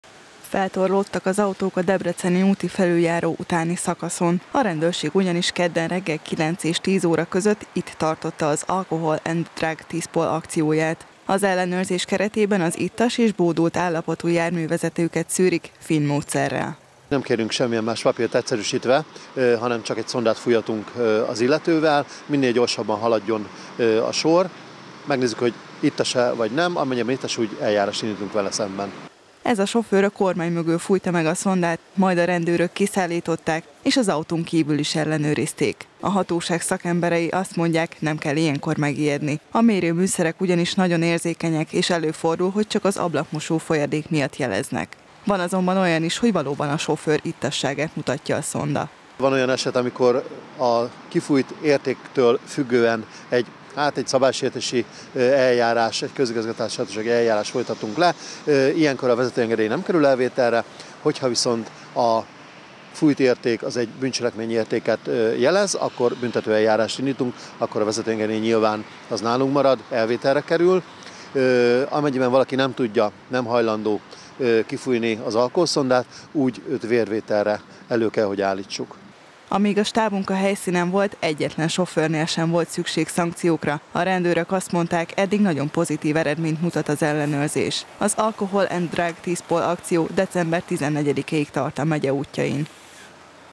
Ismét finn módszeres TISPOL
Az alkoholos befolyásoltságot figyeli a rendőrség a már zajló TISPOL akció keretein belül. A megye és az ország legforgalmasabb útszakaszain, finn módszerrel ellenőriznek a hatóság emberei. Az Alkohol and Drug akció december 9-e és 14-e között tart.